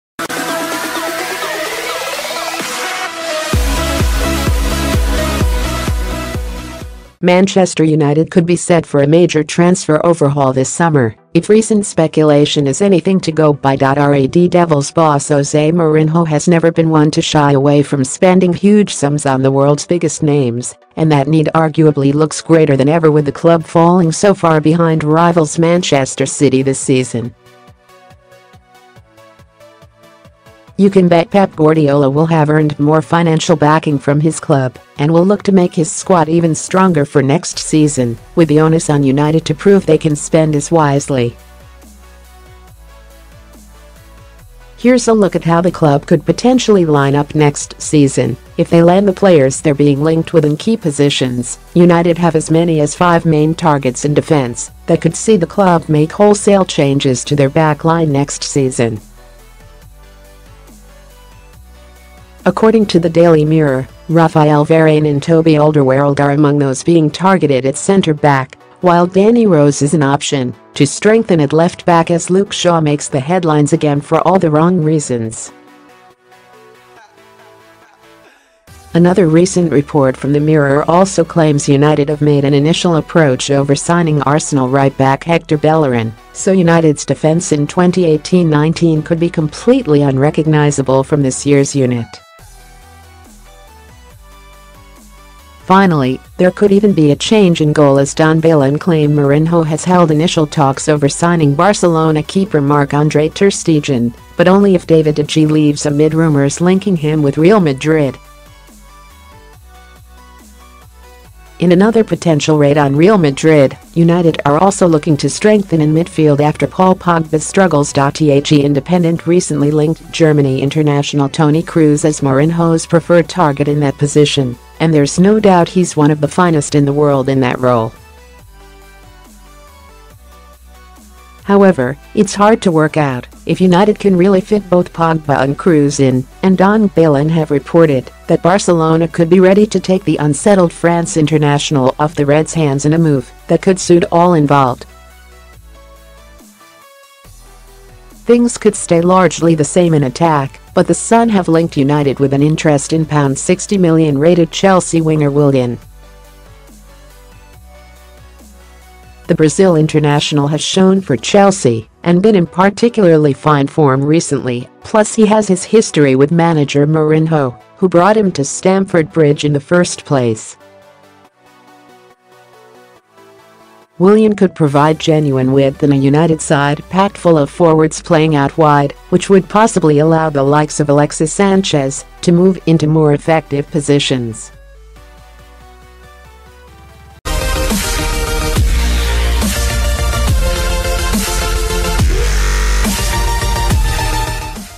Man United 2018/19 XI with Willian, Bellerin, Alderweireld transfers
Manchester United could be set for a major transfer overhaul this summer if recent speculation is anything to go by.Red Devils boss Jose Mourinho has never been one to shy away from spending huge sums on the world’s biggest names, and that need arguably looks greater than ever with the club falling